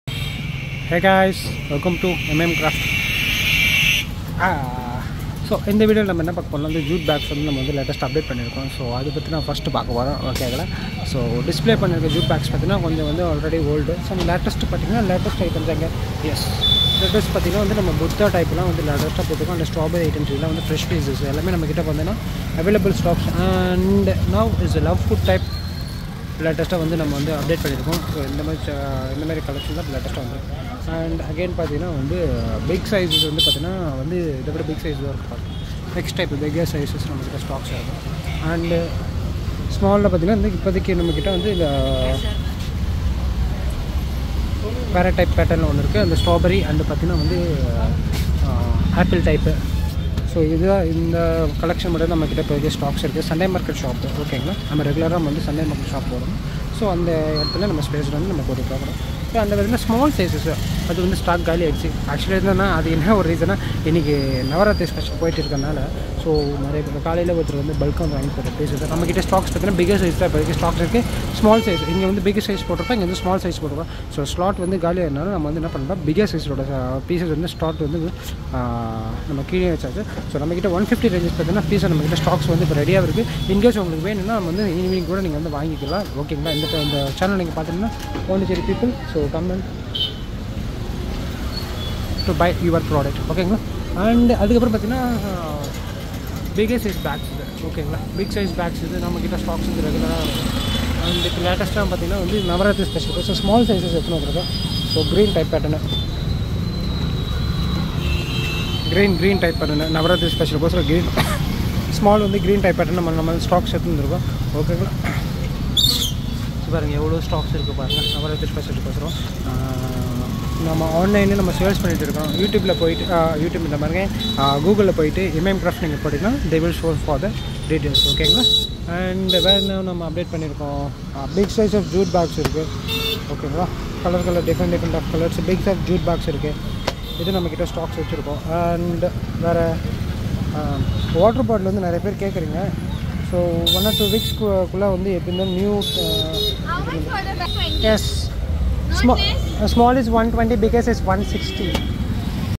Sunday market shop Mgroad pondicherry | jute and hand bags display @IMAYAMCRAFTS @Imcrshorts week 4
Sunday market shop in pondicherry Jute and hand bag display many collection are available here so if you want more details dm me..